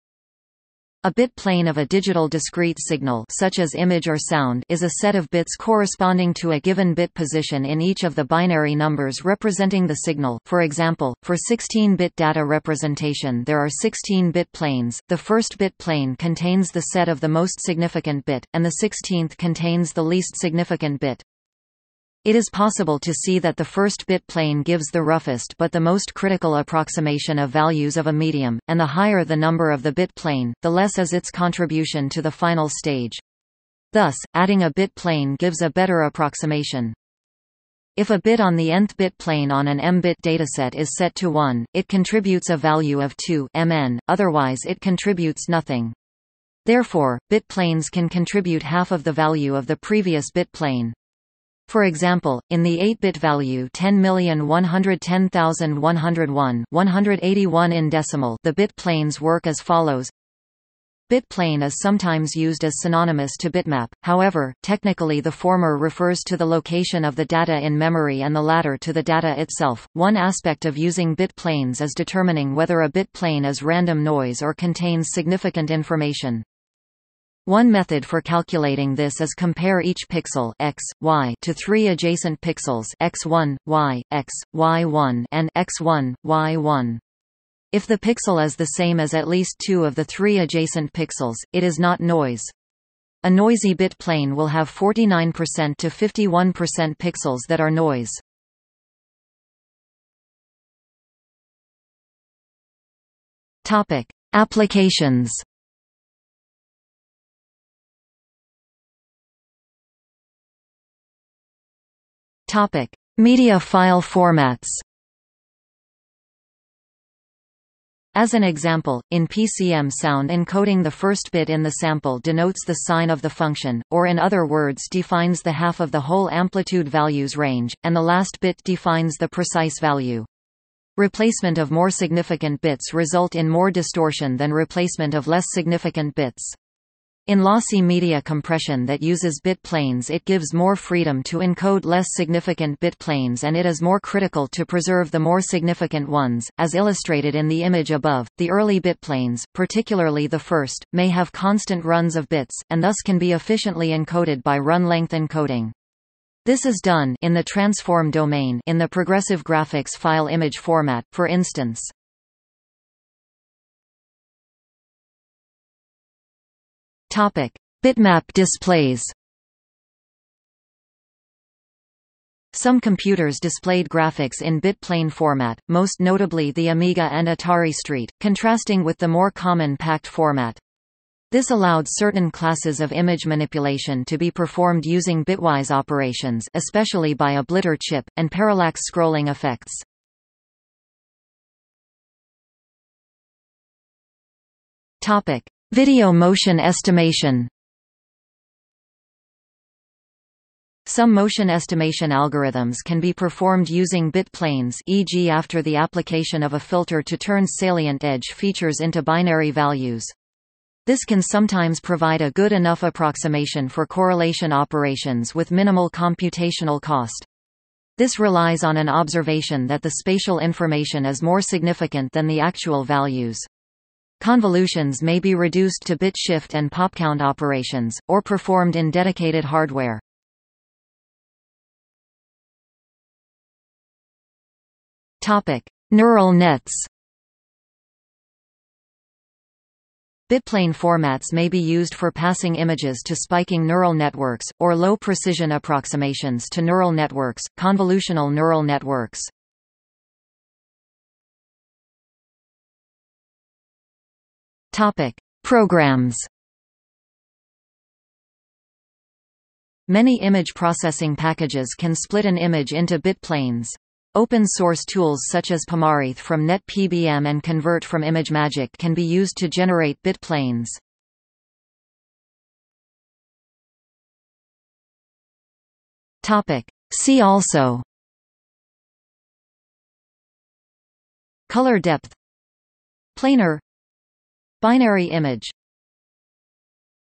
Bit plane | Wikipedia audio article
00:01:40 1 Applications
00:01:49 1.1 Media File Formats
00:02:43 1.2 Bitmap displays
00:03:11 1.3 Video Motion Estimation
00:03:51 1.4 Neural Nets
00:04:11 2 Programs
00:04:32 3 See also

- Socrates

SUMMARY
A bit plane of a digital discrete signal (such as image or sound) is a set of bits corresponding to a given bit position in each of the binary numbers representing the signal.For example, for 16-bit data representation there are 16 bit planes: the first bit plane contains the set of the most significant bit, and the 16th contains the least significant bit.
It is possible to see that the first bit plane gives the roughest but the most critical approximation of values of a medium, and the higher the number of the bit plane, the less is its contribution to the final stage. Thus, adding a bit plane gives a better approximation.
If a bit on the nth bit plane on an m-bit dataset is set to 1, it contributes a value of 2(m-n), otherwise it contributes nothing. Therefore, bit planes can contribute half of the value of the previous bit plane. For example, in the 8-bit value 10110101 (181 in decimal) the bit planes work as follows:

Bit plane is sometimes used as synonymous to Bitmap; however, technically the former refers to the location of the data in memory and the latter to the data itself.One aspect of using bit-planes is determining whether a bit-plane is random noise or contains significant information.
One method for calculating this is compare each pixel (X,Y) to three adjacent pixels (X-1,Y), (X,Y-1) and (X-1,Y-1). If the pixel is the same as at least two of the three adjacent pixels, it is not noise. A noisy bit-plane will have 49% to 51% pixels that are noise.